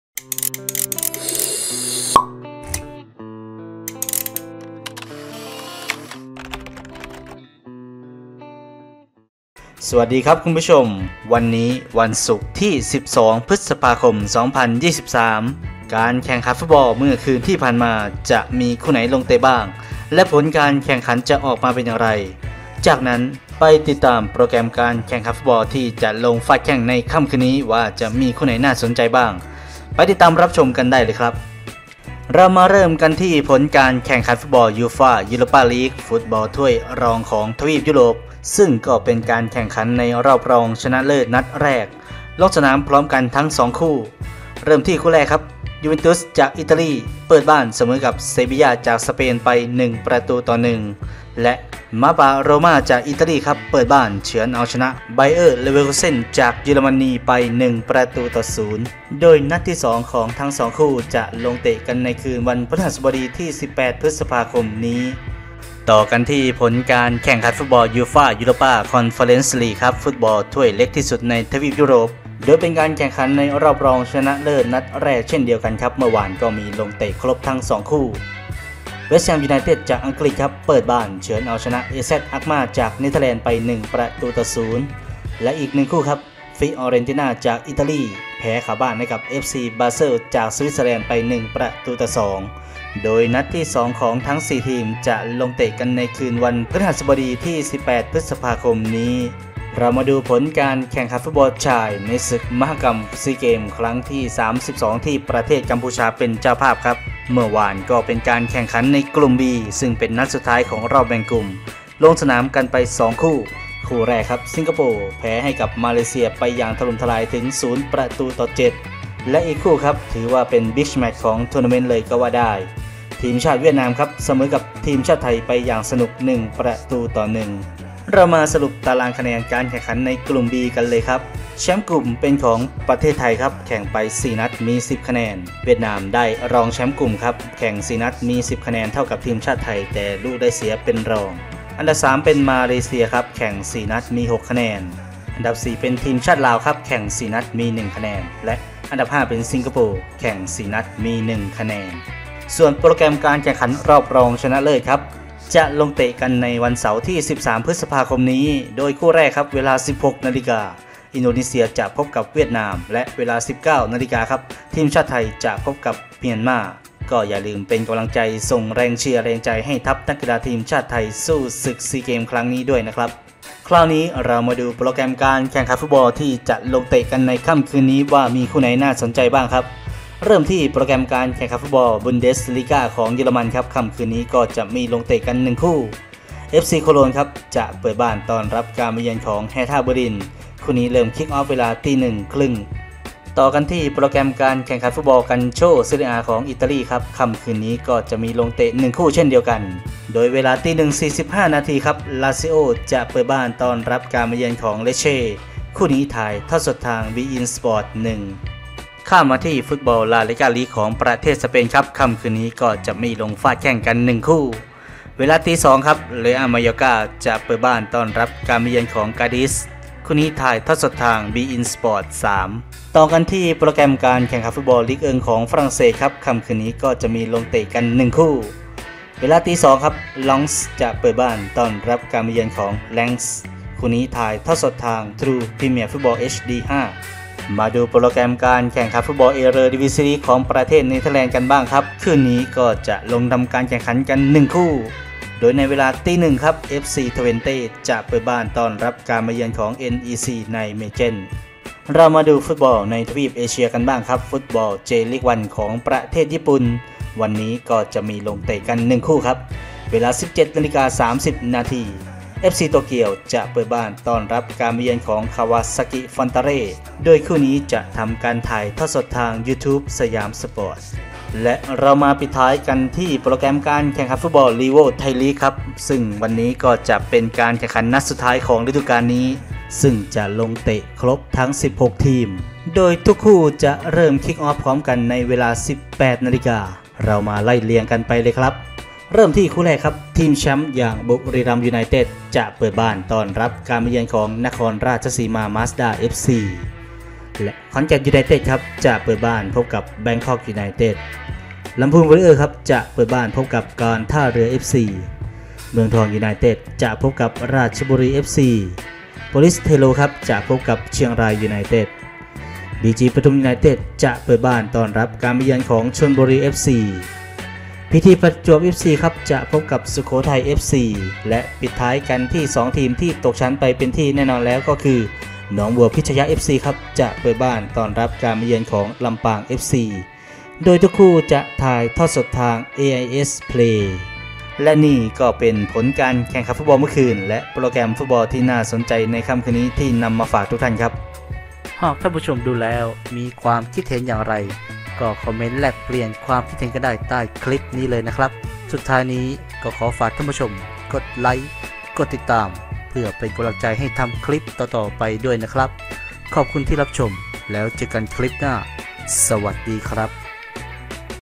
ผลบอลเมื่อคืน/โปรแกรมบอลคืนนี้/12.05.2023/ยูโรป้า/คอนเฟอเรนซ์/ซีเกมส์/บุนเดส/กัลโช่/ลาลีก้า/เจ/ไทย
และขอคนละไลค์คนละคอมเม้นต์ เพื่อเป็นกำลังใจให้ทำคลิปต่อๆ ไปด้วยนะครับ

ติดตามข่าวสารช่องทาง Facebook แฟนเพจ :

00:00 สวัสดี
00:31 ผลบอลยูโรป้าลีก
01:05 ผลบอลคอนเฟอเรนซ์ลีก
01:39 ผลบอลซีเกมส์
02:53 โปรแกรมบุนเดสลีก้า
03:12 โปรแกรมกัลโช่ซีเรียอา
03:28 โปรแกรมลาลีก้า
03:42 โปรแกรมลีกเอิง
03:57 โปรแกรมเอเรอดีวีซี
04:11 โปรแกรมเจลีก
04:31 โปรแกรมไทยลีก